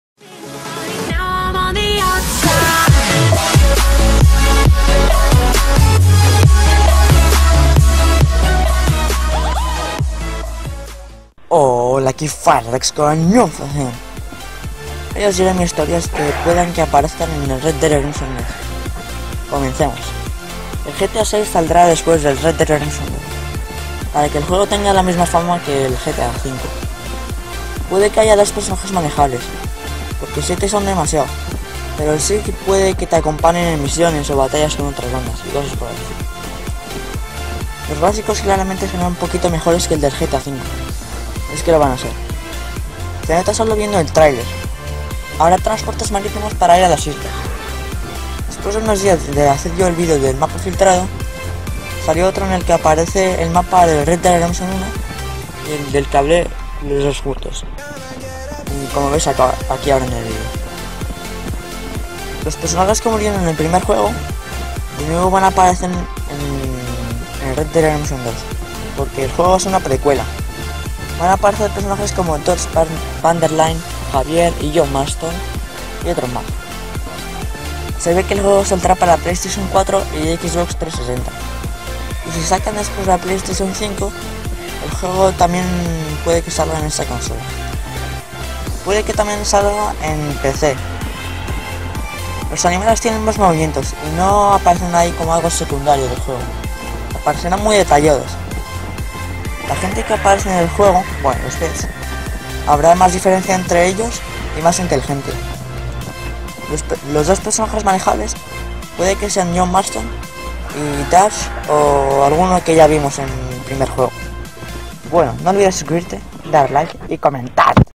MIS TEORIAS RED DEAD REDEEMPTION 2!!!!!!!!
En este video hablo sobre mis teorias sobre el Red Dead Redemption 2